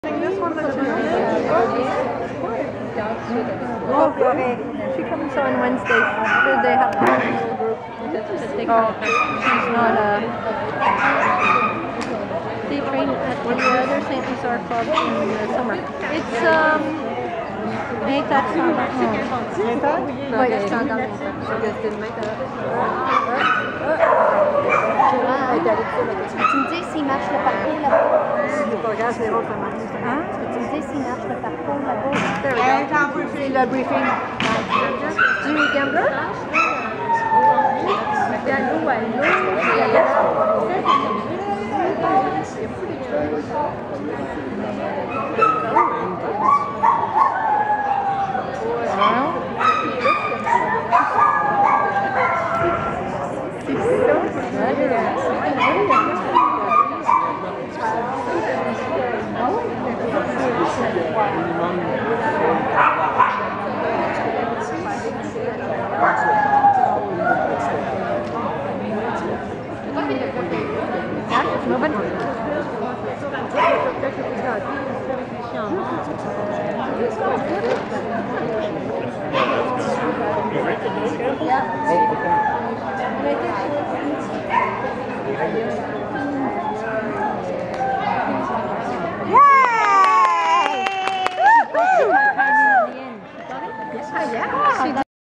Agility Yorkshire "Pudding"
Club Canin Chomedey Trial - February 21-22-23, 2014
Advanced Standard
The AADC Title Run